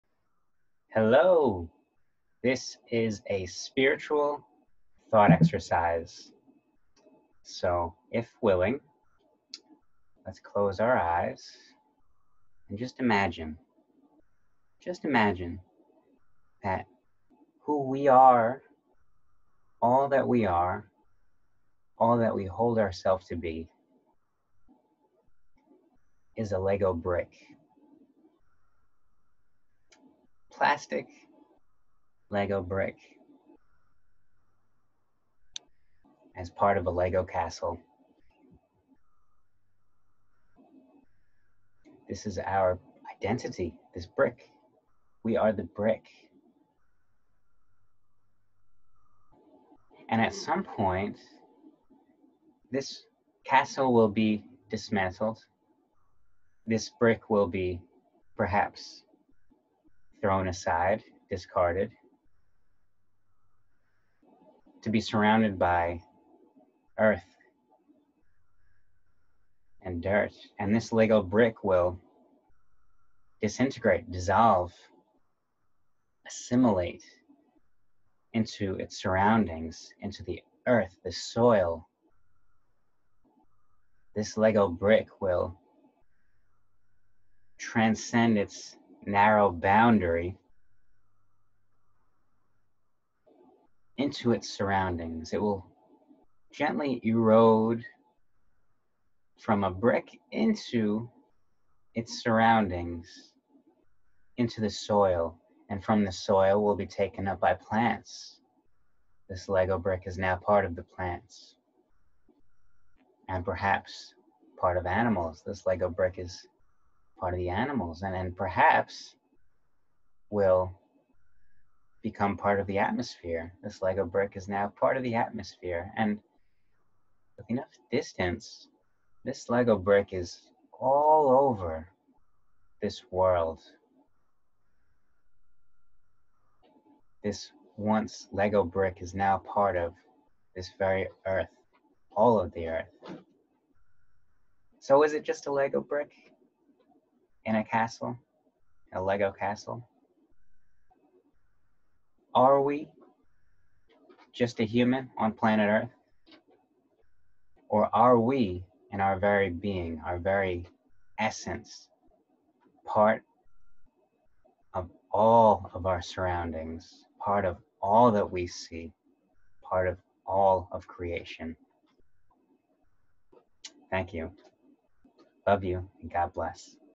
Lego Brick
Imagine your identity as...a lego brick.

As silly as this may seem, it puts our identity as a human in perspective.